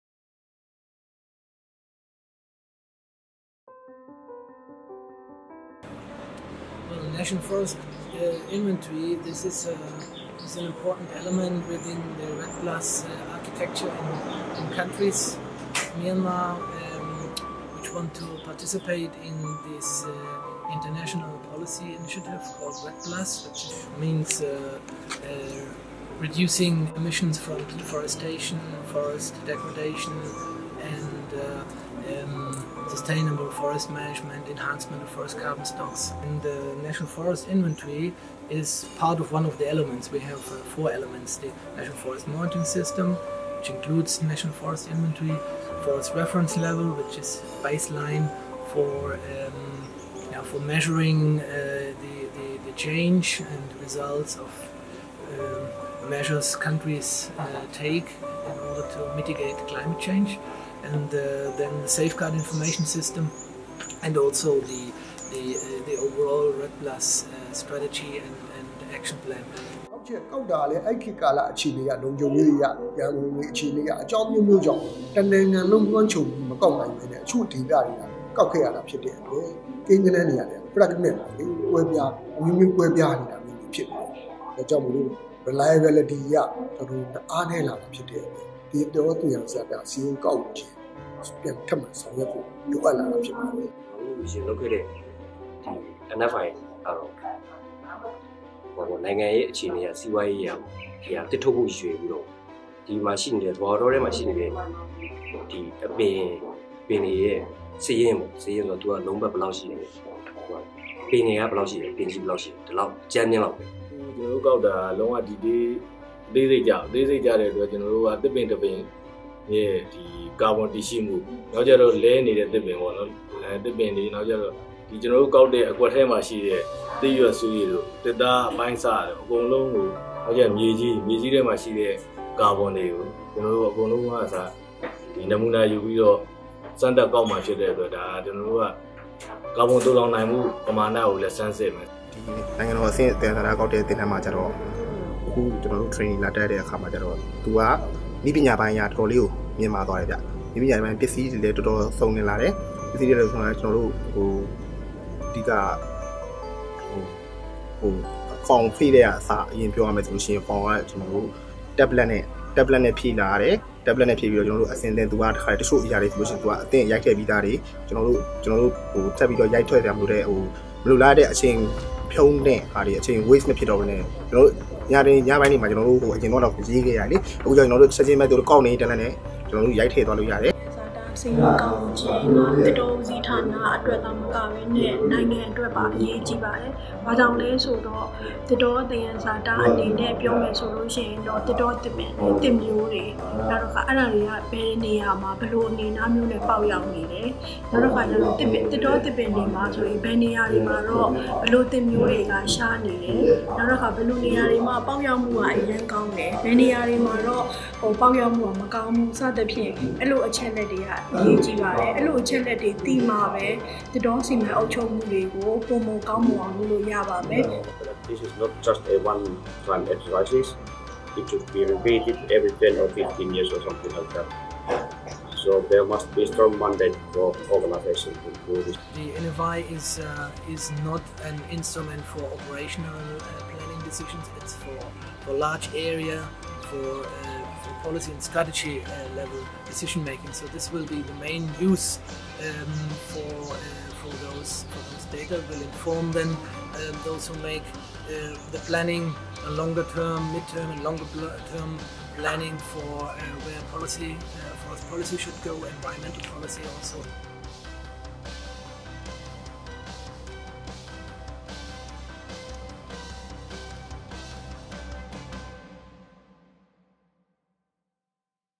Progress in Designing and Planning of a National Forest Inventory in Myanmar
Under the framework of the UN-REDD Programme in Myanmar,FAO has been supporting the Forest Department in developing a new National Forest Inventory (NFI) as an important component of its enhanced National Forest Monitoring System . FAO’s expertise globally and in the region in implementing NFIs helped the country to set up an NFI approach which is objective driven and user-oriented taking into account key information needs of relevant stakeholders as well as the increasing monitoring and reporting requirements on forests at international level. An effective field methodology was developed in 2018 which is the basis for the training and field testing carried out between February and April 2019. In this video  training participants and experts share their views on the NFI in Myanmar and talk about the crucial role of the availability of robust forest information in ensuring sound forest policy planning and evaluation for sustainable forest use as well as responding to international monitoring needs in the context of Climate Change and REDD+.